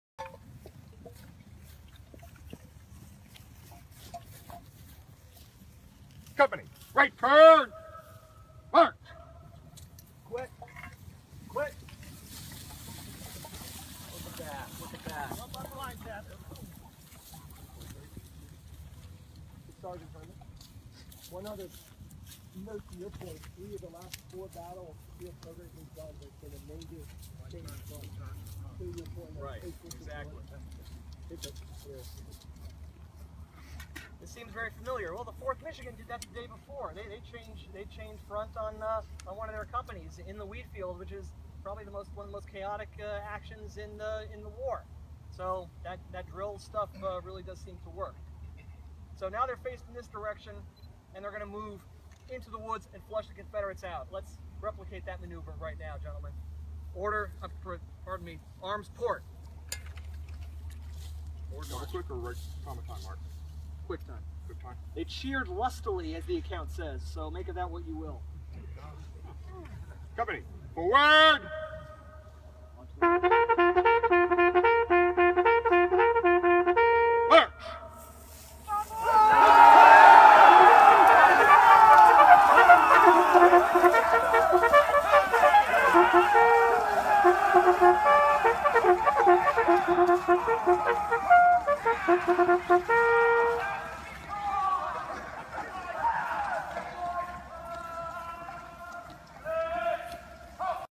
The Liberty Rifles following the footsteps of the Pennsylvania Reserves at Gettysburg, Nov. 2016.
The Liberty Rifles following the footsteps of the Pennsylvania Reserves at Gettysburg, November 2016.